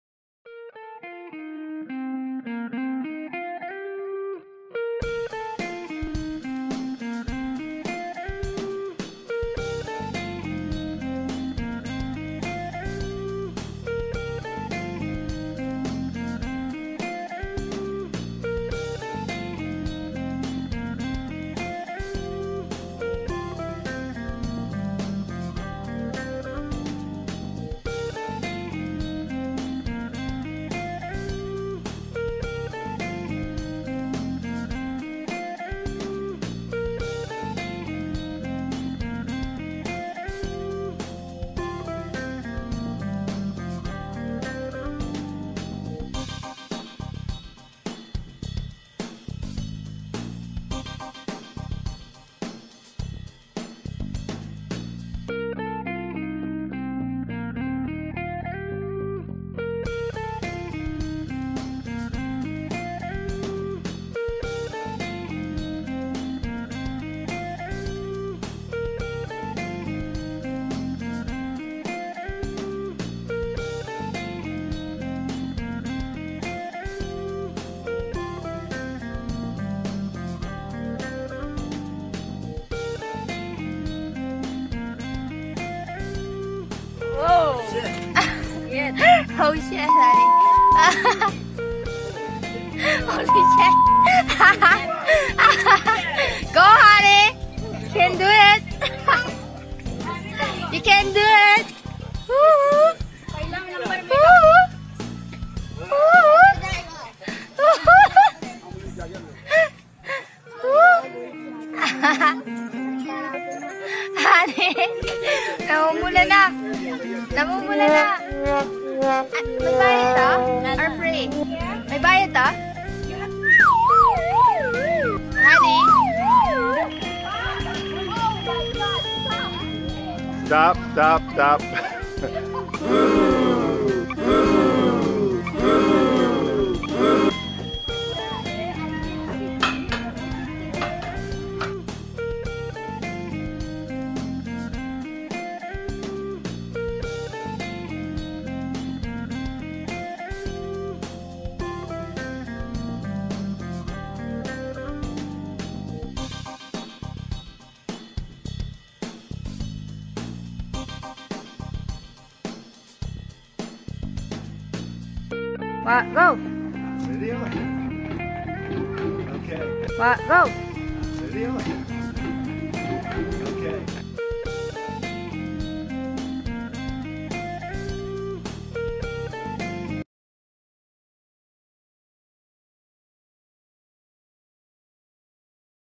Upside Down House Pattaya + Human Gyroscope.
Unique Upside Down House Pattaya and Our Human gyroscope experience.

Pretty impressive inside and Perfect for taking photos. It's a very special experience, once you are inside the house but you will feel dizzy. Other than the house there are a hedge maze and a children's playground.